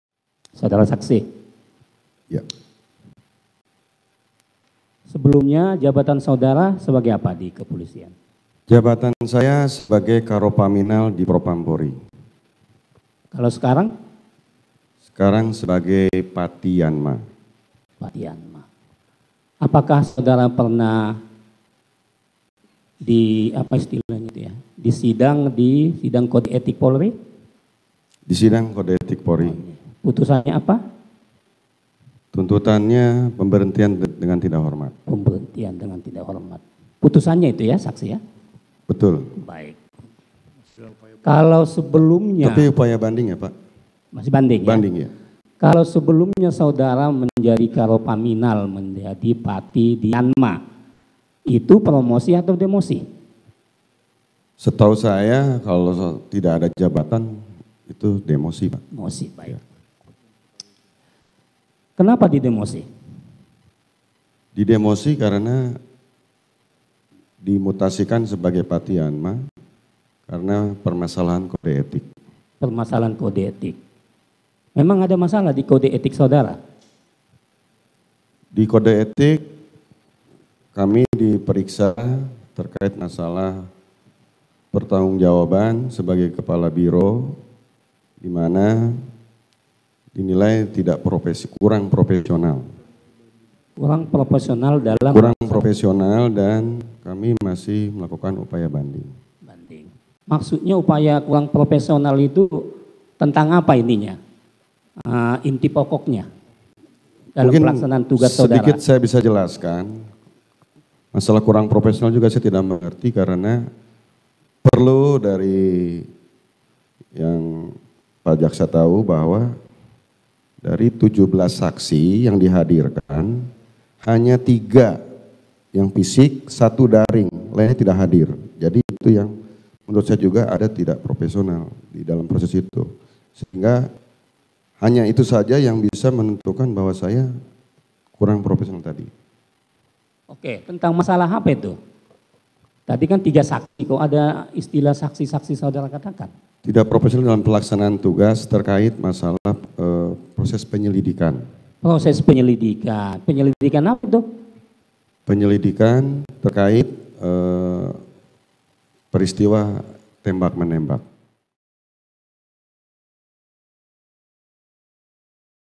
Di Tengah Sidang, Hendra Kurniawan Sebut Sidang Etik Pemecatan Dirinya Tidak Berjalan Profesional
JAKARTA, KOMPAS TV - Eks Karo Paminal Propam Polri Hendra Kurniawan sempat lontarkan kritik terhadap sidang etik pemecatan dirinya, di tengah persidangan obstruction of justice, Jumat (16/12).

Hal tersebut diucapkan Hendra saat ditanyakan jaksa terkait profesi dan yang sempat ia alami semasa di Polri.

Jaksa menanyakan apakah Hendra Kurniawan pernah jalani sidang kode etik di Polri atau tidak.

Baca Juga Terungkap! Chuck Sebut Ferdy Sambo Marah Bareskrim Polri Olah TKP di Duren Tiga Soal Kematian Yosua di

"Tuntutannya (sidang kode etik) pemberhentian dengan tidak hormat.

"Tapi upaya banding," lanjutnya.

Lalu Hendra tiba-tiba menceritakan apa yang ia pikirkan tentang sidang kode etik yang ia jalani beberapa bulan lalu.

"Sedikit saya bisa jelaskan, masalah kurang profesional juga saya tidak mengerti," ungkap Hendra.

"Dari 17 saksi yang dihadirkan, hanya 3 yang fisik 1 daring, lainnya tidak hadir. Menurut saya juga ada tidak profesional," lanjutnya.

Hari ini (16/12) sidang perkara obstruction of justice kembali digelar dengan terdakwa AKP Irfan Widyanto.

Sejumlah saksi mahkota dihadirkan hari ini, yaitu Ferdy Sambo, Hendra Kurniawan, Agus Nurpatria, dan Arif Rahman.

Irfan Widyanto merupakan salah satu terdakwa perintangan penyidikan atau obstruction of justice dalam penanganan peristiwa pembunuhan Brigadir J atau Yosua.

Sehari sebelumnya, Irfan bersaksi untuk dua terdakwa Hendra Kurniawan dan Agus Nurpatria.